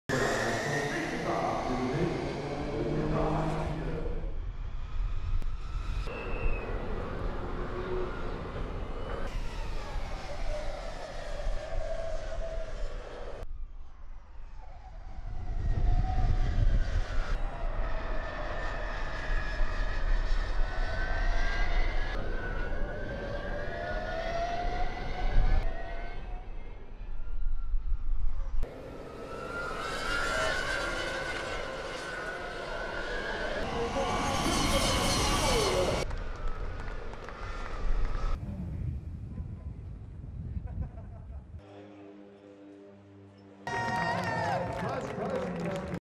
Motorsport MotoE France | Granado leaves it late to win French MotoE Grand Prix
STORYLINE:

A last lap pass gave One Energy Racing's Eric Granado victory in the MotoE French Grand Prix at Le Mans on Sunday.

The Brazilian overtook World Cup leader Alessandro Zaccone late on the final lap to clinch his first victory of the 2021 season - the second time he had overtaken the Italian in a frantic finale..

Zaccone finished second but was demoted to third for exceeding track limits on the final lap - that meant Mattia Casadei was promoted from third to second.

A thrilling race saw the first seven riders finish within one second of each other.

# EricGranado